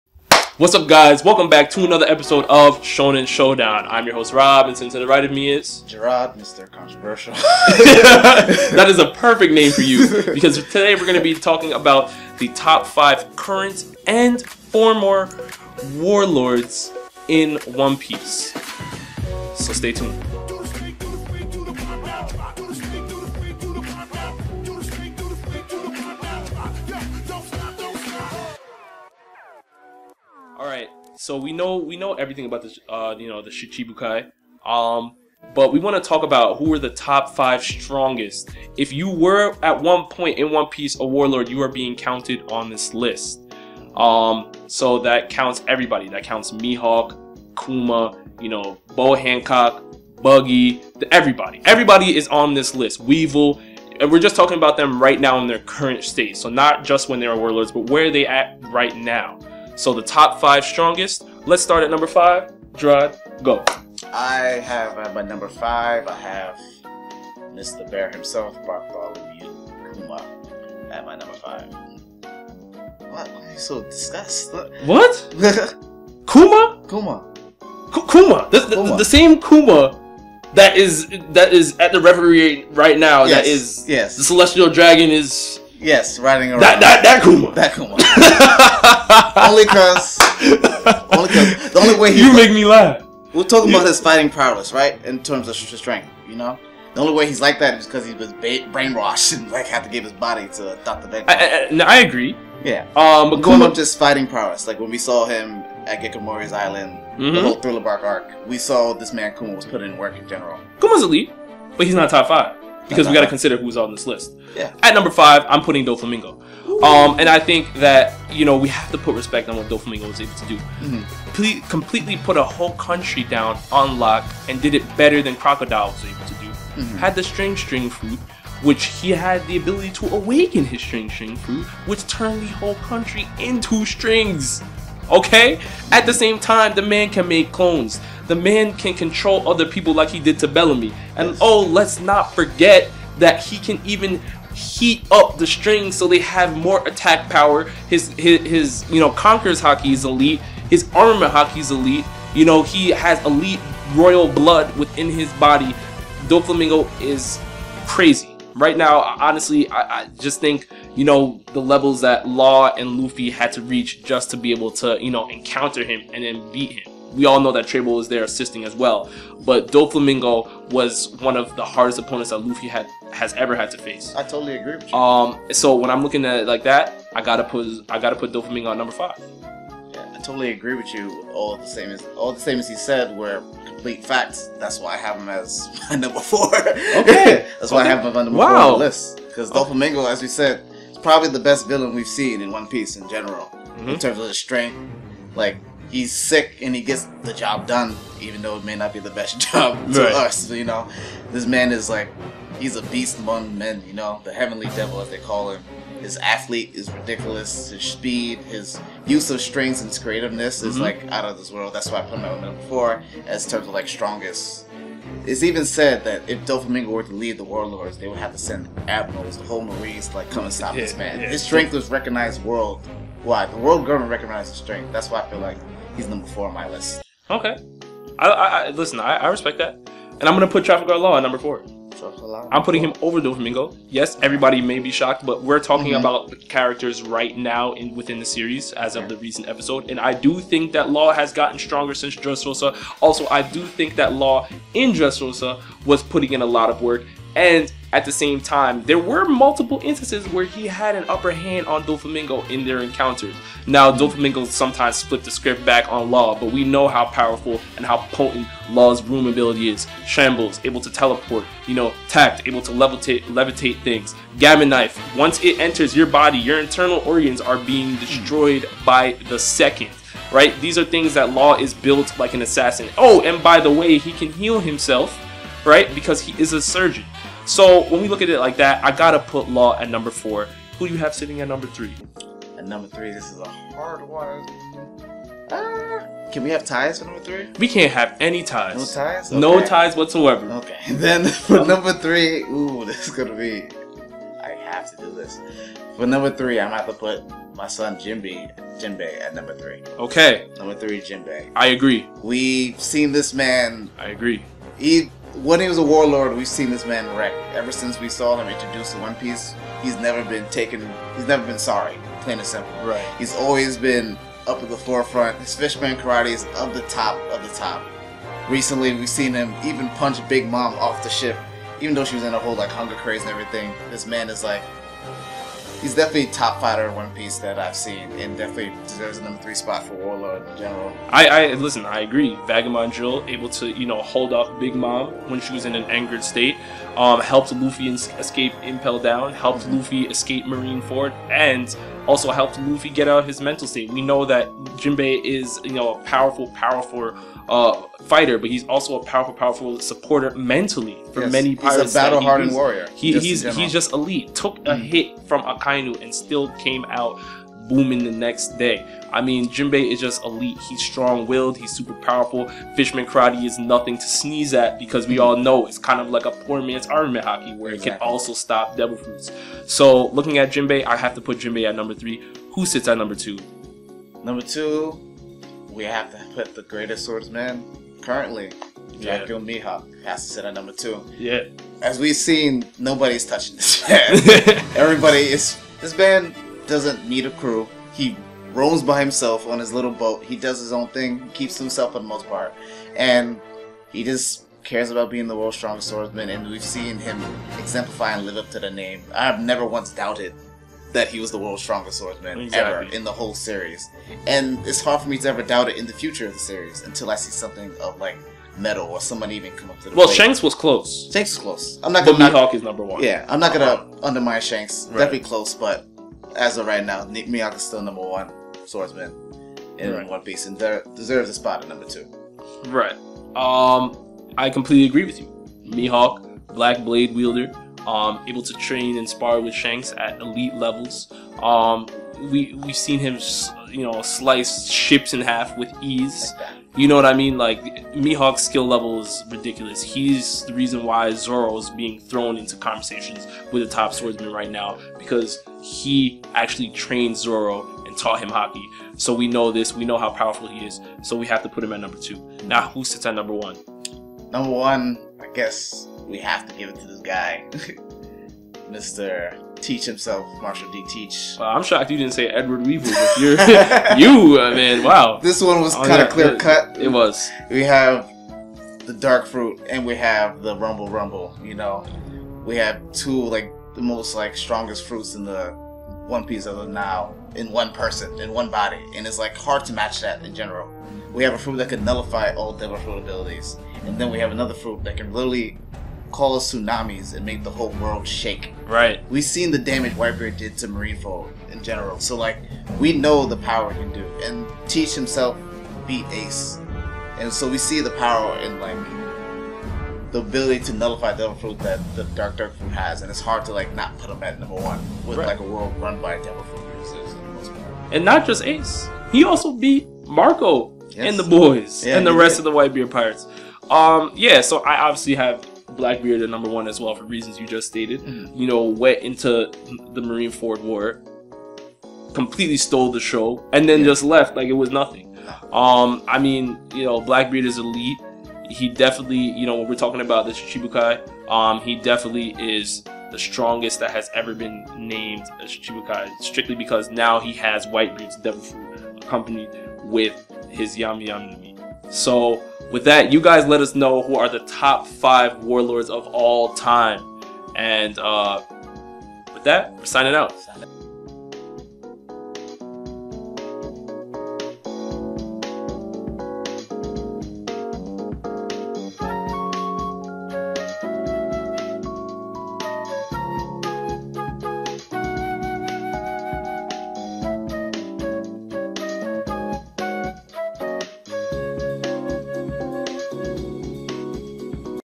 Top 5 Warlords (Shichibukai) of All Time in One Piece!!
*Apologies for the excess background noise.*

We are discussing the Top 5 Warlords of All-Time after the abolition of the warlord system. Find out here!!!